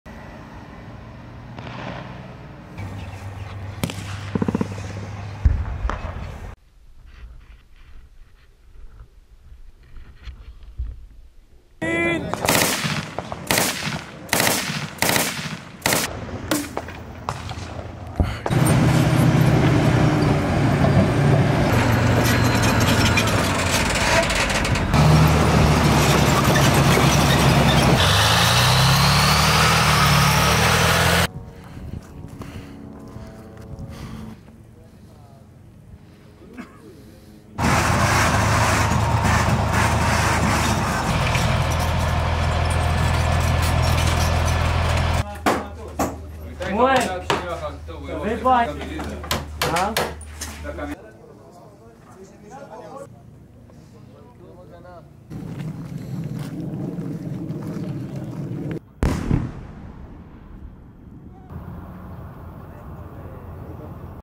צפו בפעילות הכוחות הקרקעיים ברצועת עזה | מלחמת 'חרבות ברזל'
בפעילות הקרקעית ברצועת עזה חוסלו מחבלים רבים של ארגון הטרור חמאס; הושמדו מאות מטרות צבאיות נוספות

כוחות משולבים של צה"ל, בהכוונת שב"כ ואמ"ן, תקפו כ-300 מטרות במהלך היממה האחרונה, ביניהן פירי עמדות שיגור נ"ט ורקטות, פירי מנהרות ומתחמים צבאיים של ארגון הטרור חמאס.

במהלך פעילויות הכוחות הקרקעיים, הכוחות ניהלו מספר קרבות עם חוליות מחבלים שירו לעבר הכוחות טילי נ"ט או מקלעים.

הכוחות חיסלו מחבלים והכווינו כוחות אווירים על מנת להשמיד מטרות ותשתיות טרור בזמן אמת.

קרדיט דוצ